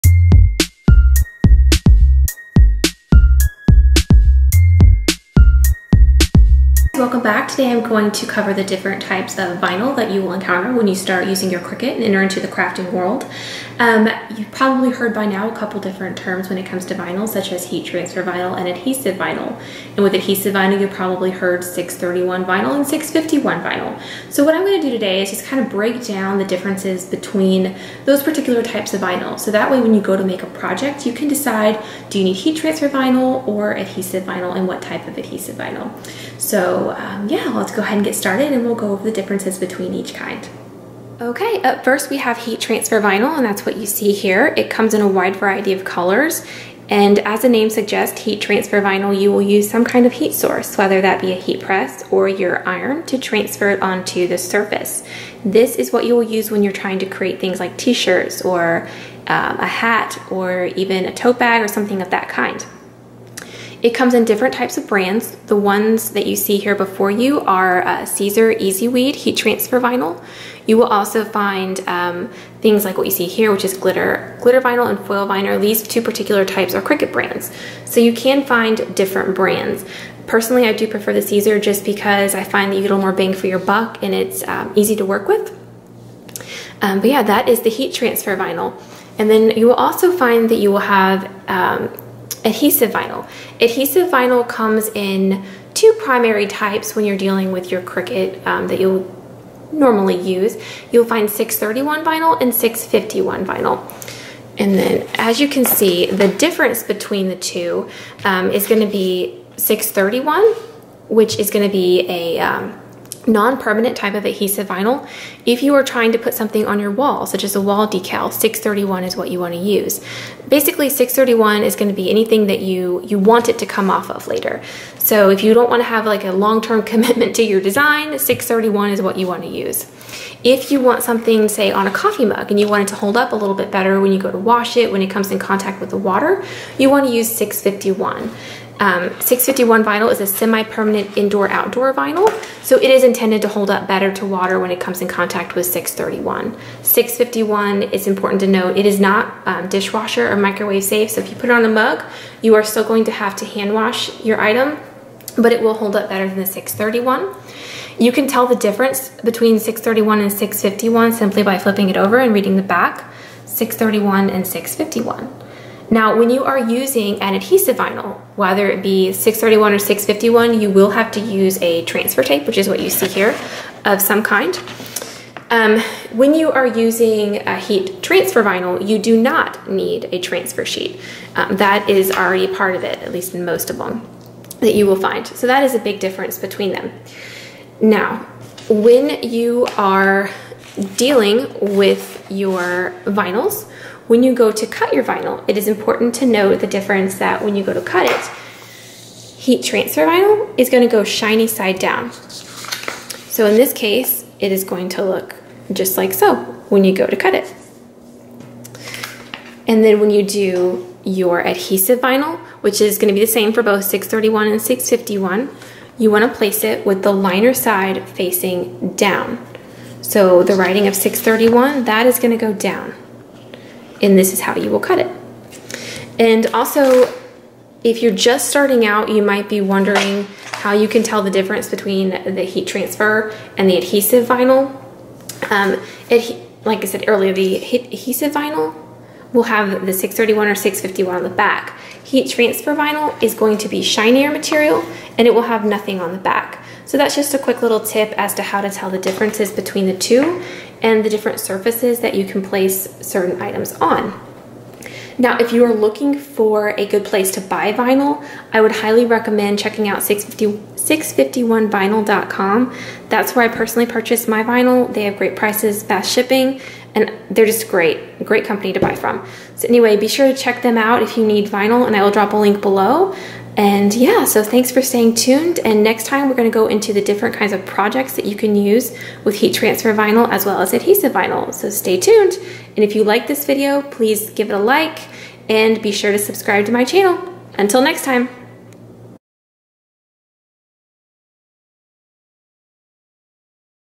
Difference between 631 Vinyl and 651 Vinyl and Heat Transfer Vinyl
This is a crash course of the differences of 631 vinyl and 651 vinyl and what the difference is between adhesive vinyl and heat transfer vinyl. Learn what type of vinyl to use for different projects and which side to place down on the cutting mat when cutting your vinyl. ALL of these topics and more are covered in the video!